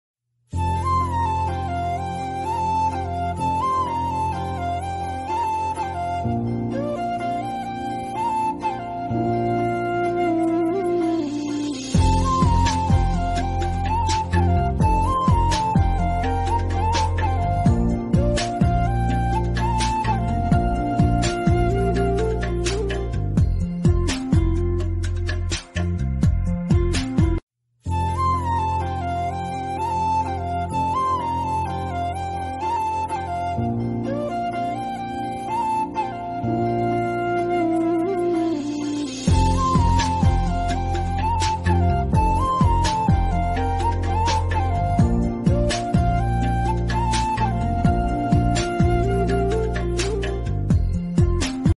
Saint of the Day - Saint Genevieve : January 3
St. Geneviève (born c. 422, Nanterre-died c. 500, Paris)
Patron saint of Paris, who allegedly saved that city from the Huns

St.Genevieve is known as the Patron saint of Disasters and Fever

Day-By-Day with the Saints in 30 seconds - Deus amat te